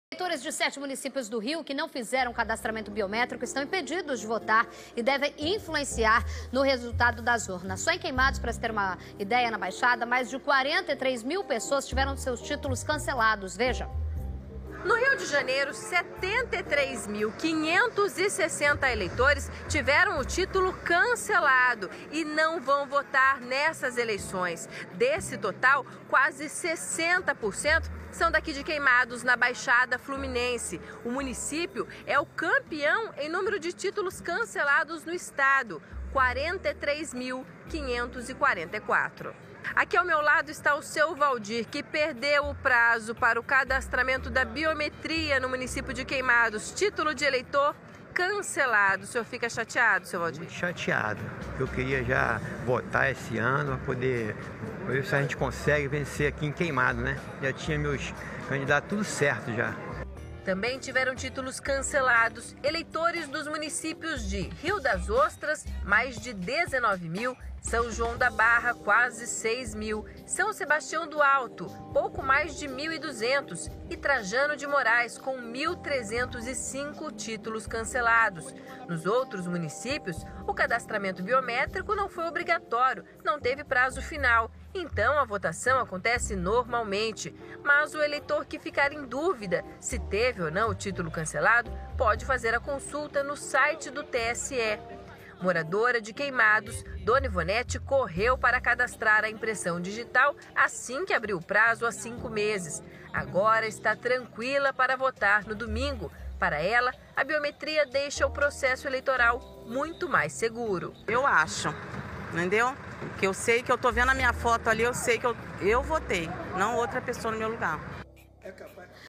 Eleitores que não fizeram o cadastramento biométrico têm títulos cancelados em 7 municípios do RJ
Eleitores de sete municípios do Rio que não fizeram o cadastramento biométrico estão impedidos de votar, o que deve influenciar o resultado das urnas. Só em Queimados, na Baixada Fluminense, mais de 43 mil pessoas tiveram seus títulos cancelados. Isso representa quase metade do número de eleitores do município.